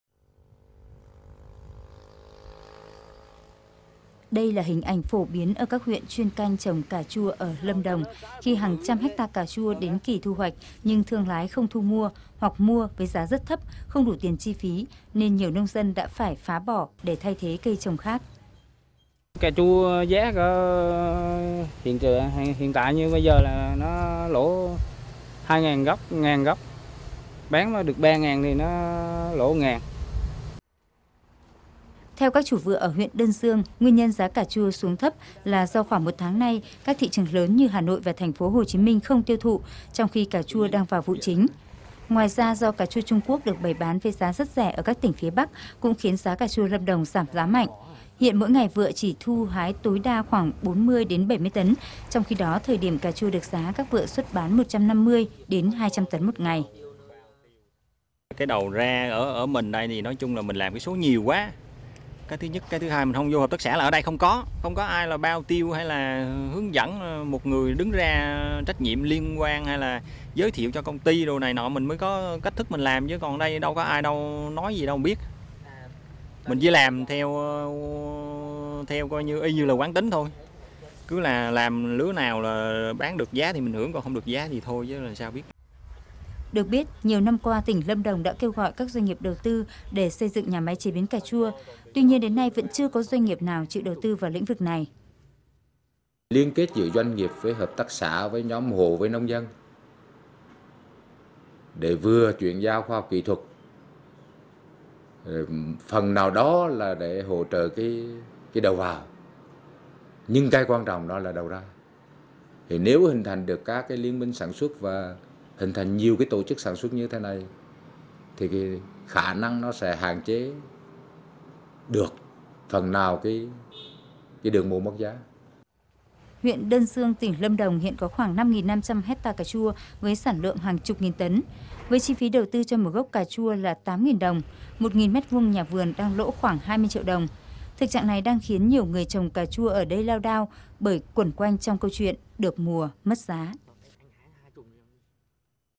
Hàng trăm tấn cà chua ở Lâm Đông bị đổ bỏ
Liên tiếp những vụ mùa cà chua gần đây người dân tại các huyện Đơn Dương, Đức Trọng (tỉnh Lâm Đồng) lâm vào cảnh khó khăn khi giá cà chua xuống thấp, giá cả bấp bênh khiến nhiều nhà vườn phải mang cà chua đi đổ bỏ. Hiện tại, giá cà chua thu mua tại vườn chỉ khoảng 500-1000 đồng/kg. Thực trạng này đang kiến người trồng cà chua tại đây hoang mang lo lắng.